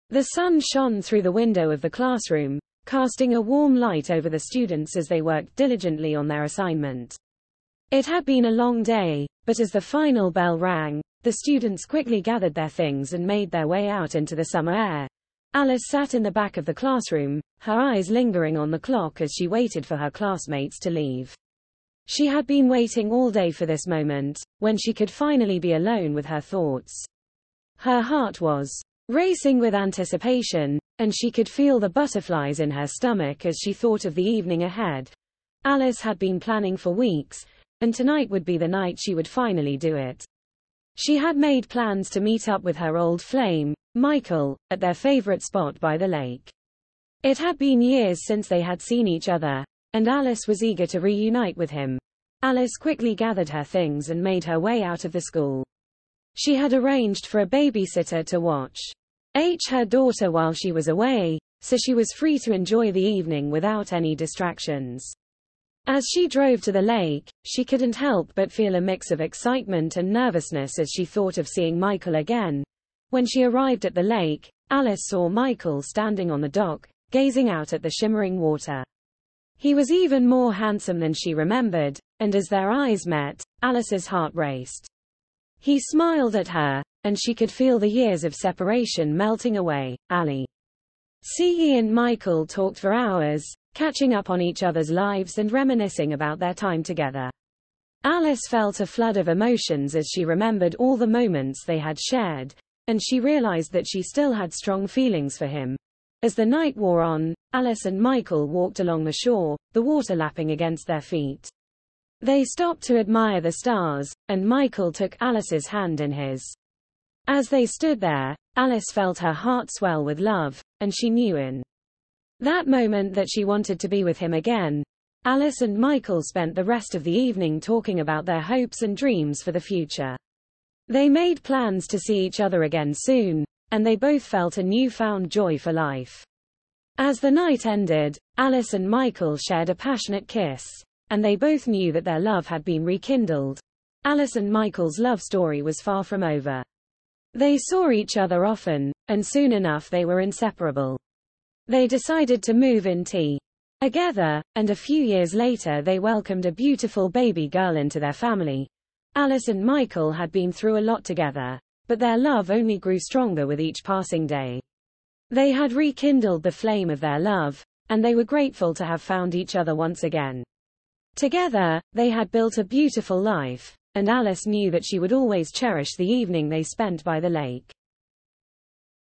A Love Rekindled
The sun shone through the window of the classroom, casting a warm light over the students as they worked diligently on their assignment. It had been a long day, but as the final bell rang, the students quickly gathered their things and made their way out into the summer air.Alice sat in the back of the classroom, her eyes lingering on the clock as she waited for her classmates to leave. She had been waiting all day for this moment, when she could finally be alone with her thoughts. Her heart was racing with anticipation, and she could feel the butterflies in her stomach as she thought of the evening ahead.Alice had been planning for weeks, and tonight would be the night she would finally do it. She had made plans to meet up with her old flame, Michael, at their favorite spot by the lake. It had been years since they had seen each other, and Alice was eager to reunite with him.Alice quickly gathered her things and made her way out of the school. She had arranged for a babysitter to watch her daughter while she was away, so she was free to enjoy the evening without any distractions. As she drove to the lake, she couldn't help but feel a mix of excitement and nervousness as she thought of seeing Michael again.When she arrived at the lake, Alice saw Michael standing on the dock, gazing out at the shimmering water. He was even more handsome than she remembered, and as their eyes met, Alice's heart raced. He smiled at her, and she could feel the years of separation melting away.Alice and Michael talked for hours, catching up on each other's lives and reminiscing about their time together. Alice felt a flood of emotions as she remembered all the moments they had shared, and she realized that she still had strong feelings for him.As the night wore on, Alice and Michael walked along the shore, the water lapping against their feet. They stopped to admire the stars, and Michael took Alice's hand in his. As they stood there, Alice felt her heart swell with love, and she knew in that moment that she wanted to be with him again.Alice and Michael spent the rest of the evening talking about their hopes and dreams for the future. They made plans to see each other again soon, and they both felt a newfound joy for life. As the night ended, Alice and Michael shared a passionate kiss, and they both knew that their love had been rekindled.Alice and Michael's love story was far from over. They saw each other often, and soon enough they were inseparable. They decided to move in together, and a few years later they welcomed a beautiful baby girl into their family.Alice and Michael had been through a lot together, but their love only grew stronger with each passing day. They had rekindled the flame of their love, and they were grateful to have found each other once again. Together, they had built a beautiful life, and Alice knew that she would always cherish the evening they spent by the lake.